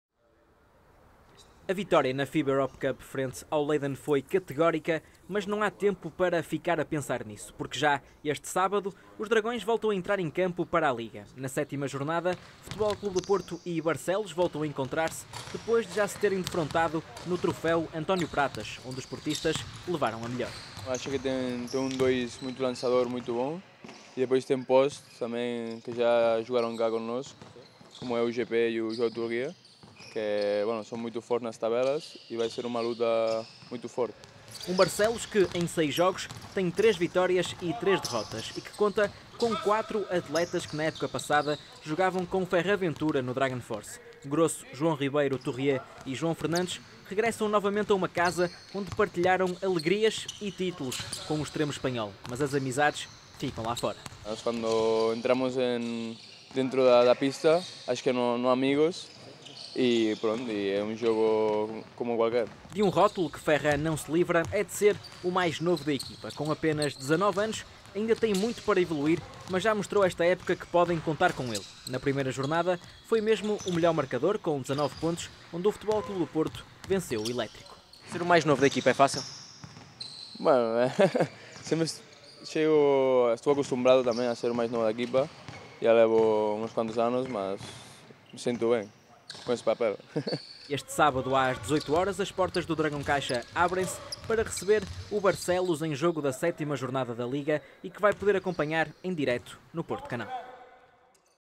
Basquetebol: Ferrán Ventura (antevisão FC Porto-Barcelos, LPB, 7.ª j., 06/11/15)
Menos de um dia depois de derrotar o ZZ Leiden (77-51) para a Taça da Europa da FIBA, o FC Porto começou a preparar a recepção ao Barcelos, que volta a visitar o Dragão Caixa depois da derrota a contar para o Troféu António Pratas. Na antevisão do jogo da sétima jornada da Liga, marcado para este sábado, às 18h00, e com transmissão em directo no Porto Canal, Ferrán Ventura perspectivou outro tipo de desafios, até porque as duas equipas agora “conhecem-se melhor”.